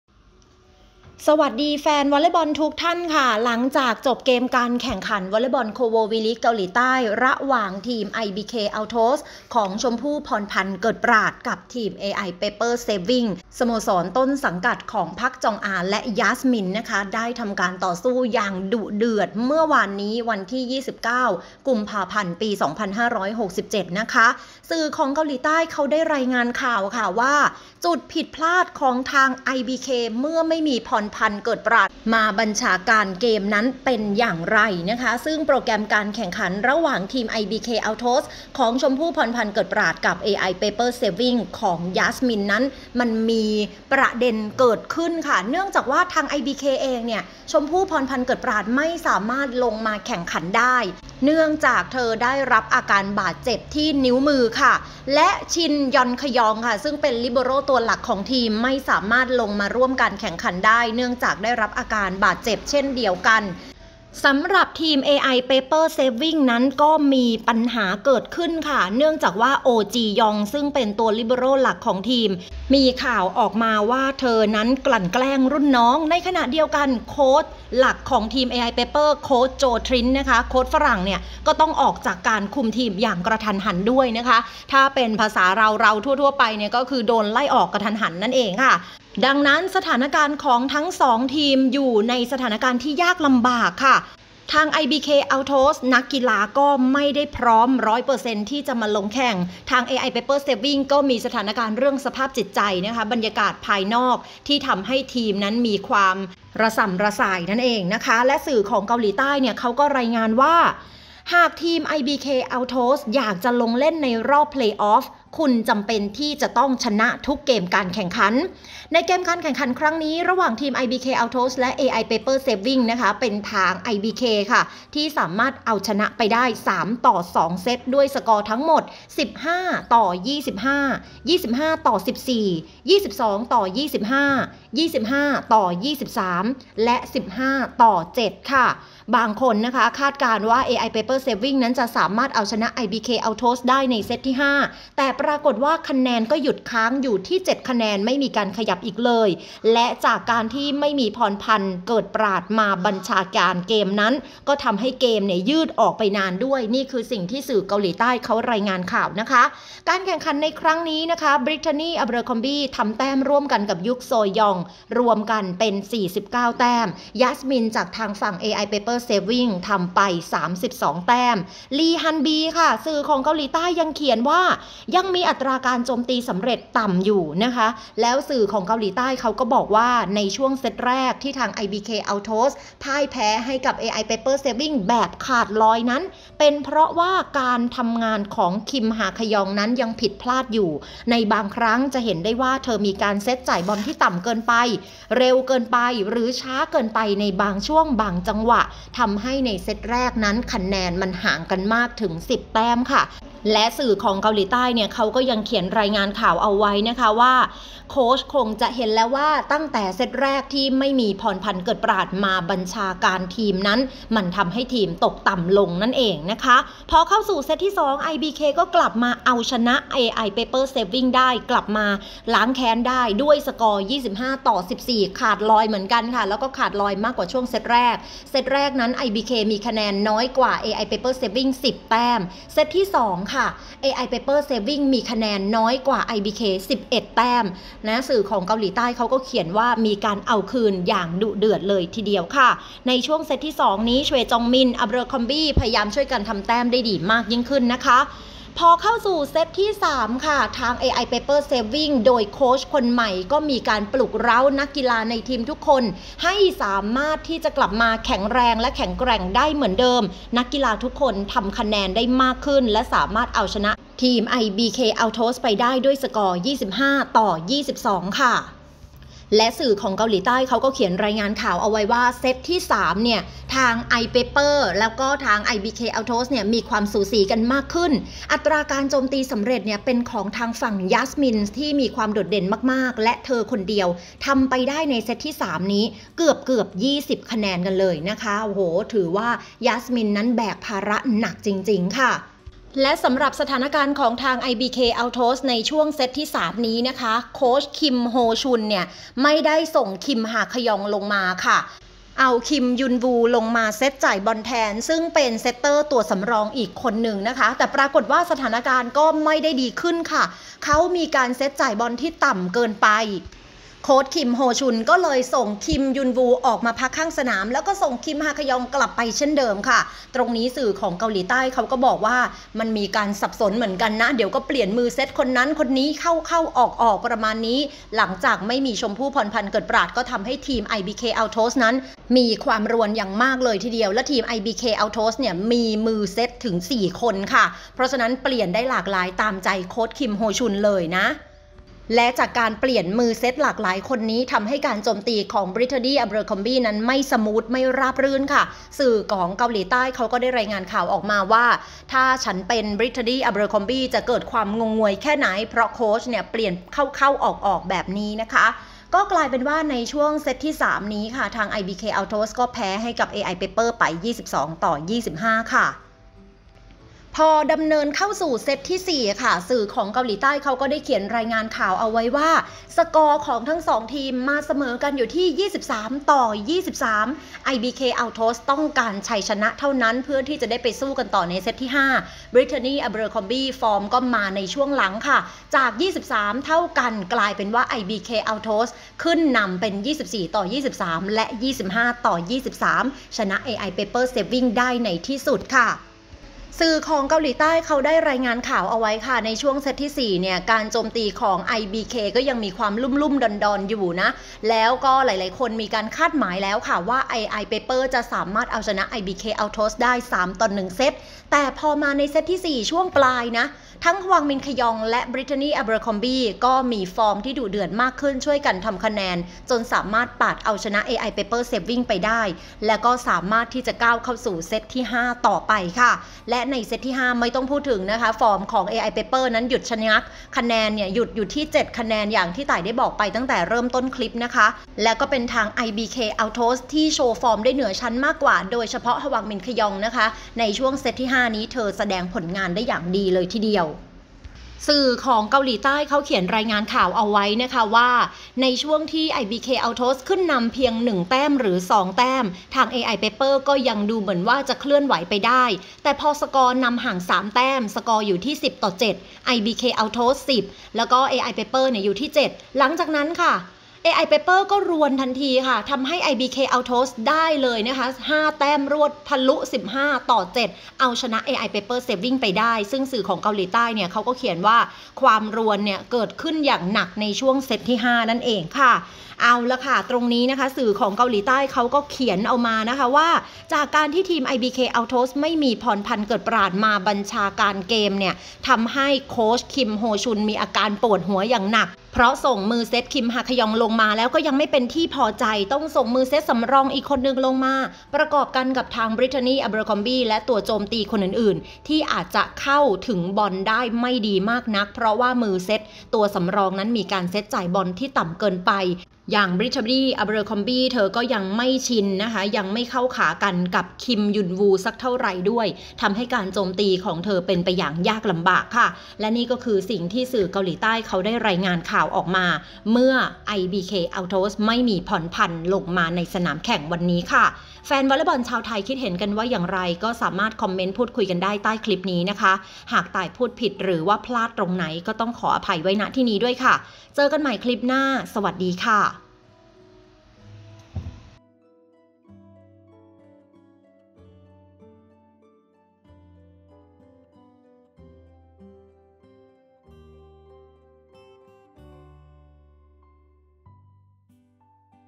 สื่อเกาหลีคิดเห็นอย่างไร...เมื่อไม่มีชมพู่อยู่ในทีม ข้อผิดพลาดของIBKสื่อมองว่ายังไง punchเชียร์กีฬา
ยินดีต้อนรับเข้าสู่ชาแนลของต่ายน่ะค่ะ
มาเชียร์กีฬาไปด้วยกันค่ะ